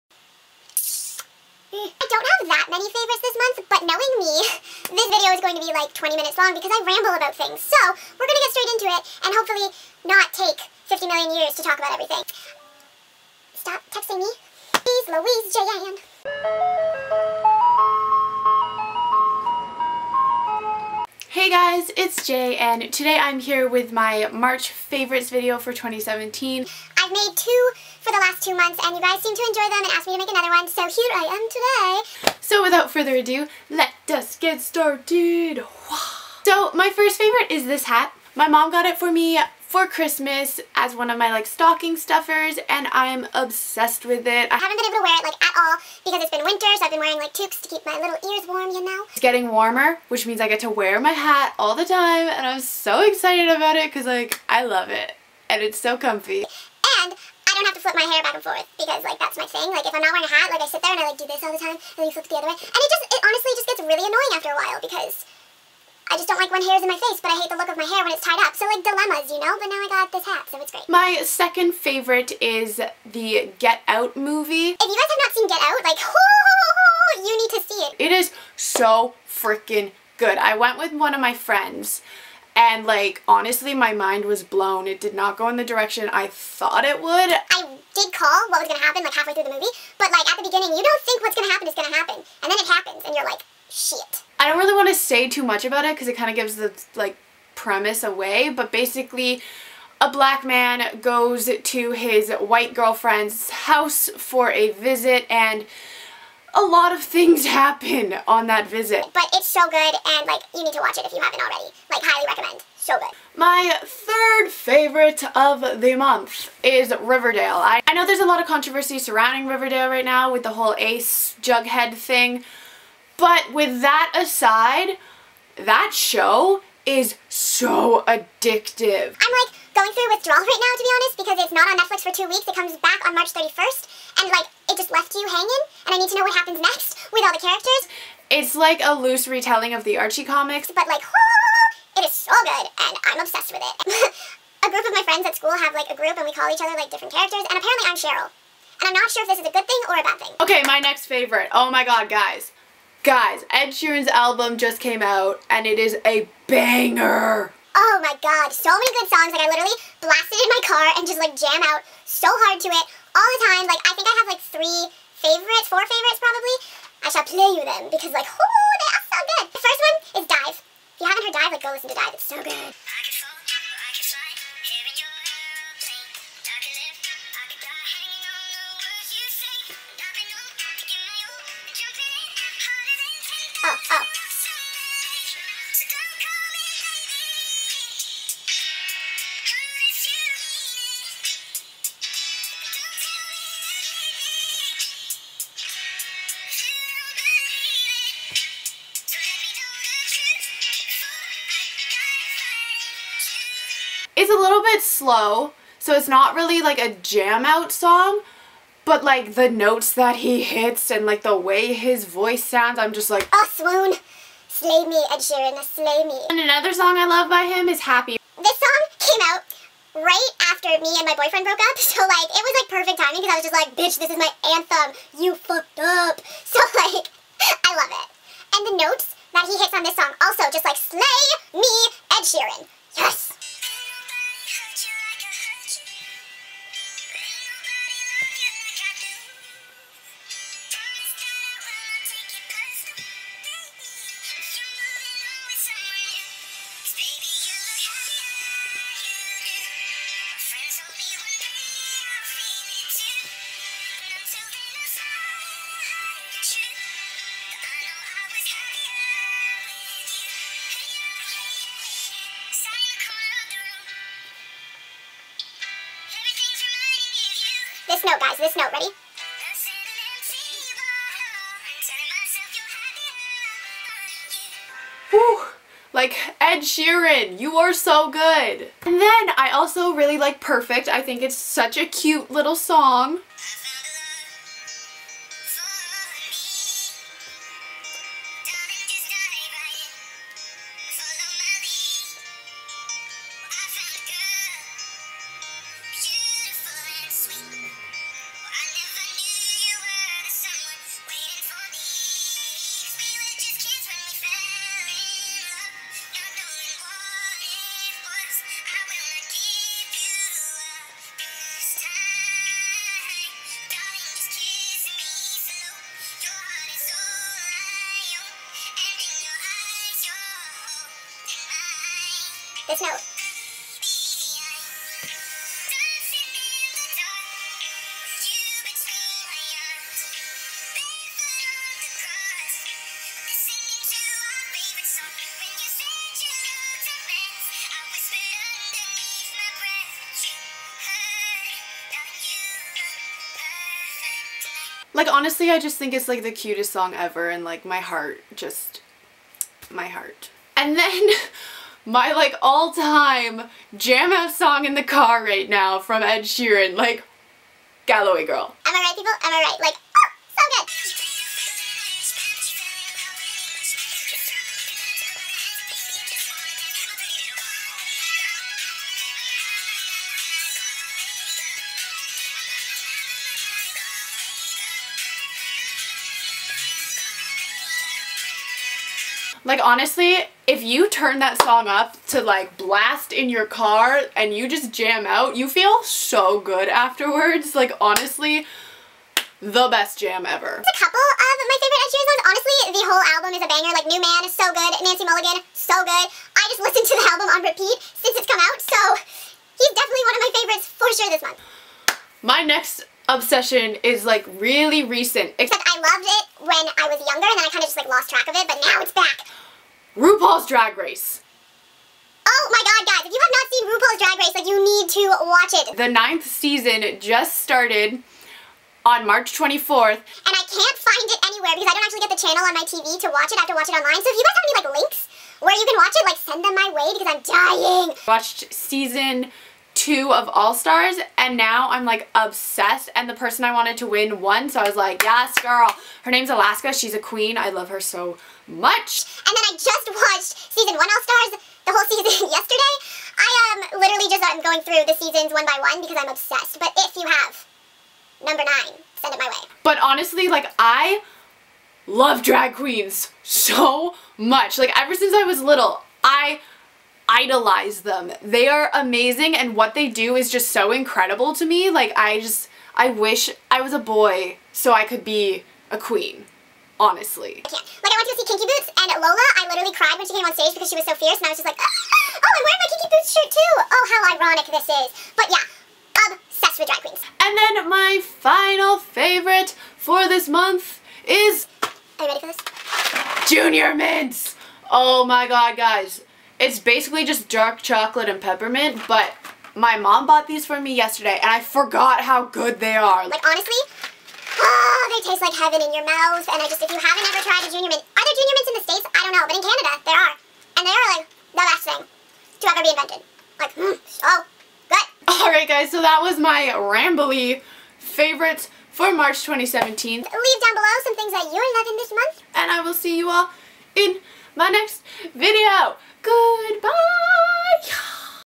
MARCH FAVOURITES | 2017
I talk a lot, I am so sorry, I just really like things. But in all seriousness, SEND ME LINKS FOR RUPAUL!!!